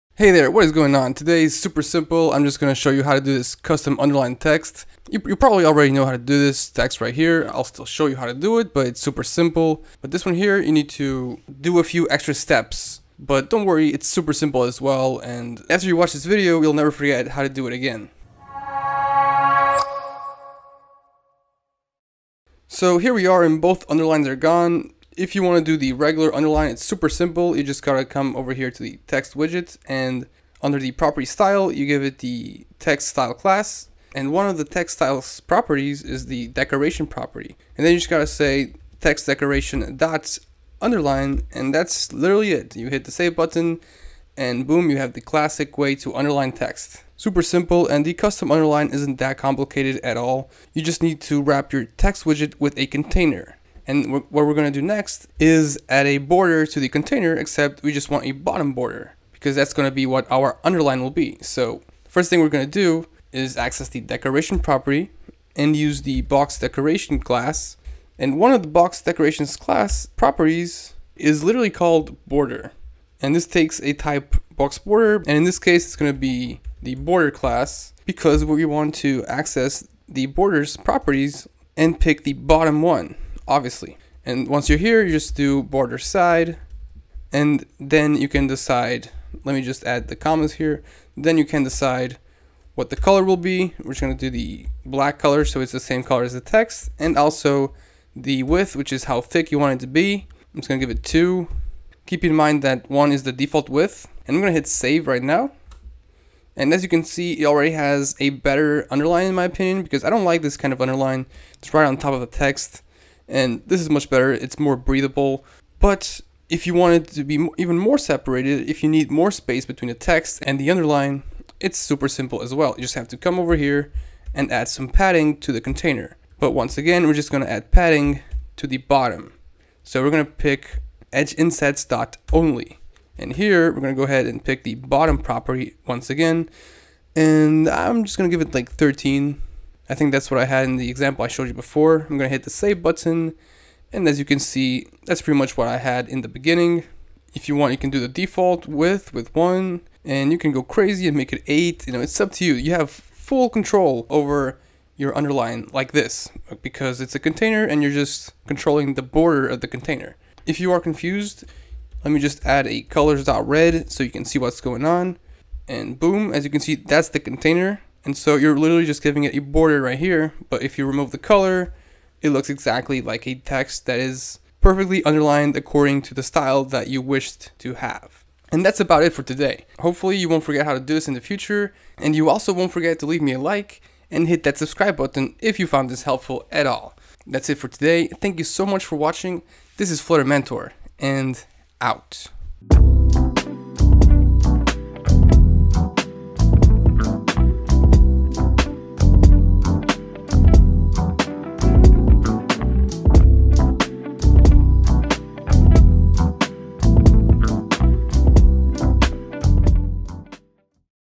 Flutter - How To Add A Custom Underline To Text (with space)
Adding a regular underline under a text is quite simple, but what if you want to add an underline with space between the text and the line? It's not that hard and I'll take you through exactly how to do it in this video. All you've got to do is wrap your Text widget with a Container and then add only a bottom border to the Container. Consequently, you'll end up with a text with an underline that you can control how apart from the text it is!

00:00 - Intro
00:22 - How To Apply Regular Underline
00:48 - How To Apply Custom Underline
01:55 - How To Add More Space
02:58 - Like & Subscribe
03:07 - Flutter Mentor Out